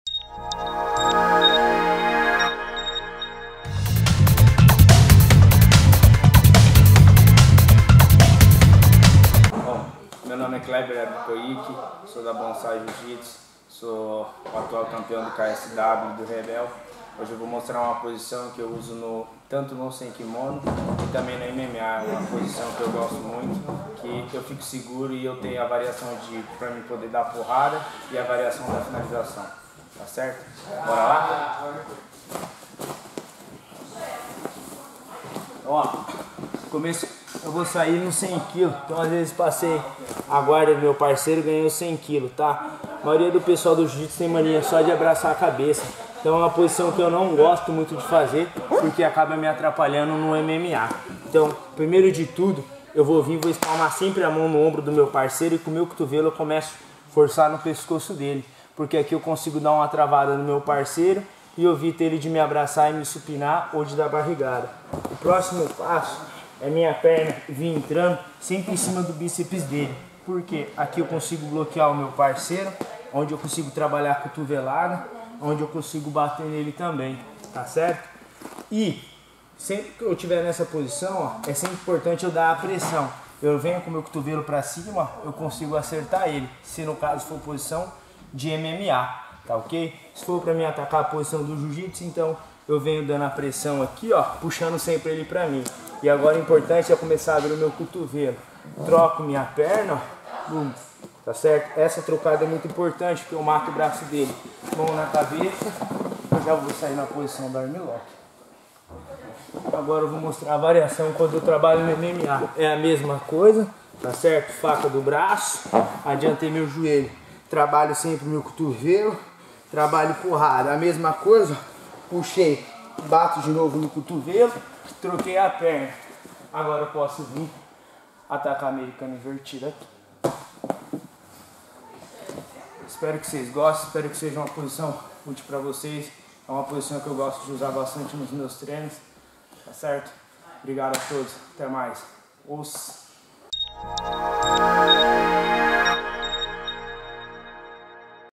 クレベル・コイケのMMAとグラップリングで使えるテクニック Tecnica Usada no MMA e Sem kimono por Kleber Koike
ヨーロッパ最大の総合格闘技イベントKSWフェザー級王者クレベル・コイケによる、MMAやグラップリングで使えるテクニック。
O campeao KSW Kleber Koike vai ensinar tecnica usada no MMA e Sem kimono.